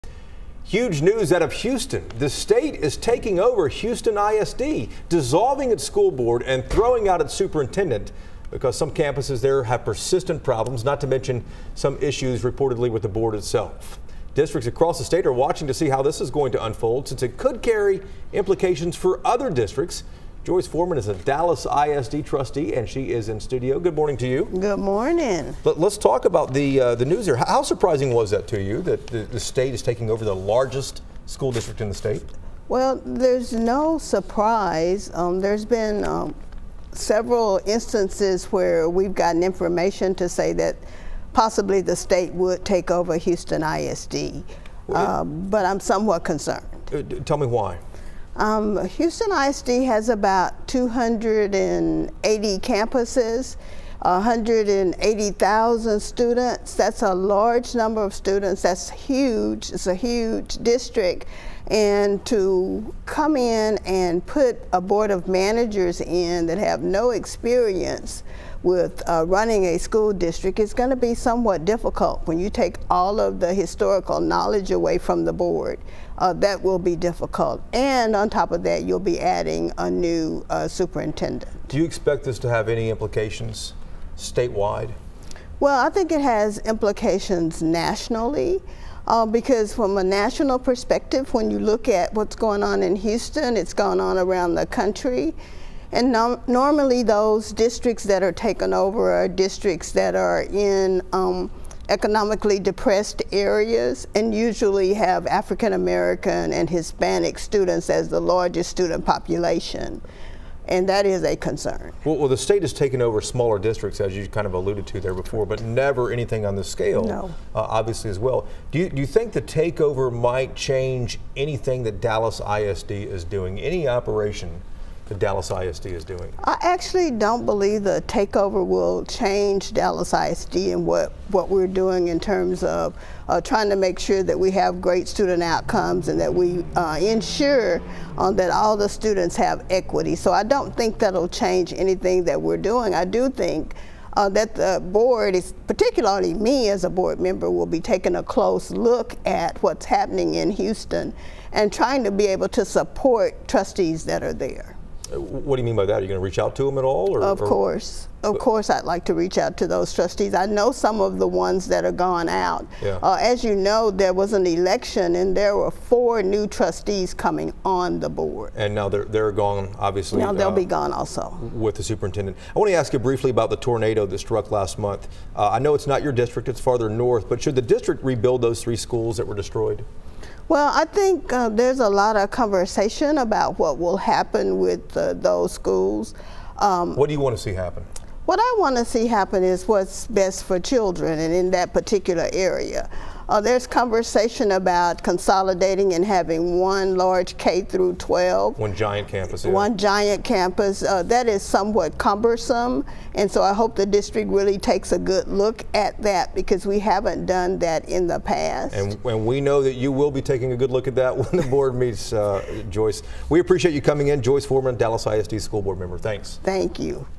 Inside Texas Politics: State to take over Houston ISD
In shocking news out of Houston last week, the state announced it is taking over Houston ISD. The Texas Education Agency will dissolve the board and remove its superintendent. Dallas ISD Trustee Joyce Foreman joined host Jason Whitely to discuss what implications this could have for other school districts.